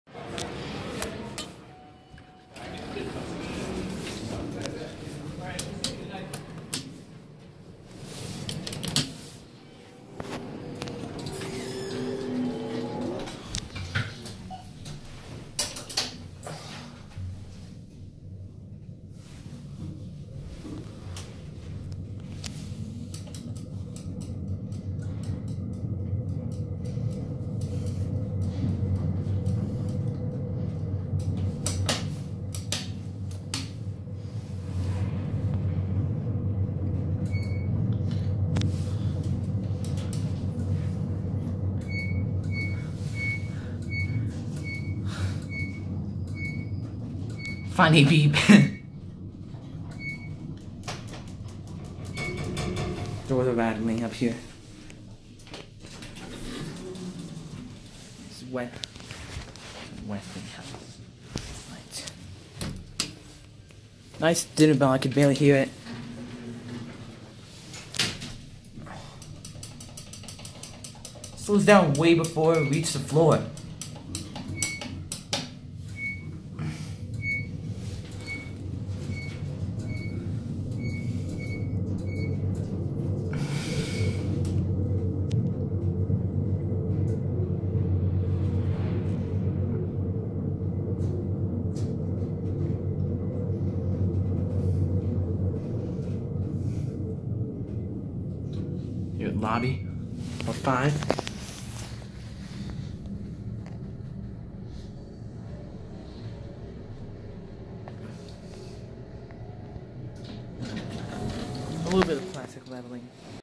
OG Take: Modded 1969 Westinghouse Selectomatic High Rise Elevators @ Safeco Plaza, Seattle WA
This thing accelerates and decelerates very slowly! This looks to be a Northern Mod. Before the Columbia Center opened, this was Seattle's tallest building from 1969 to 1985. Original Version.

Taken 11 December 2018

Installed 1969

Original Fixtures: Westinghouse AE